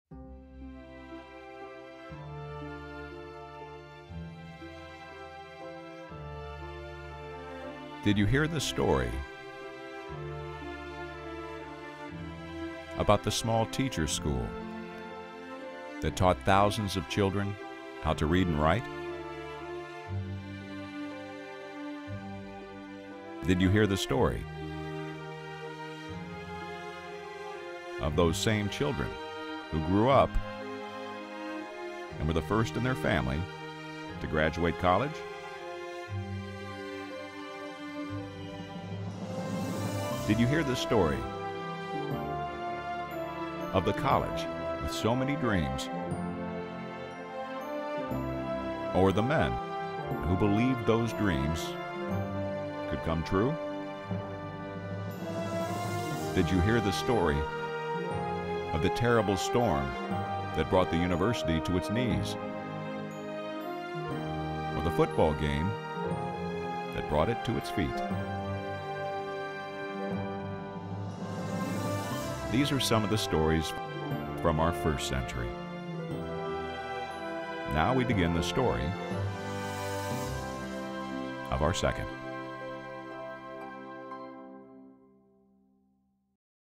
The Second Century Campaign
As we venture into the next hundred years, the Second Century Campaign will provide East Carolina with the means to deliver on the promise of the future.